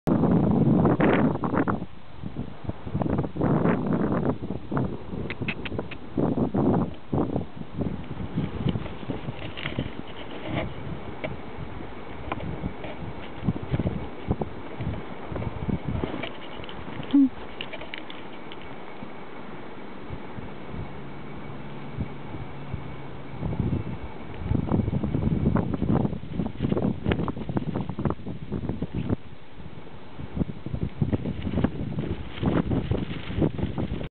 Adorable Standard Poodle Puppy experiences the Snow
Standard Poodle Running in the snow | Indy loves to play and run in the snow.  It was very windy. He buries his head in the snow all the time. He is 11 months old in this video.

Pet Products that I love

🔸Social Media 🔸